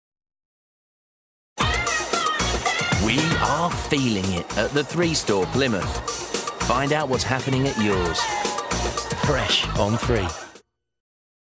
3 moile plymouth
3 Mobile In Store Dance Plymouth choreography done by Natricia Bernard and directed by Huse.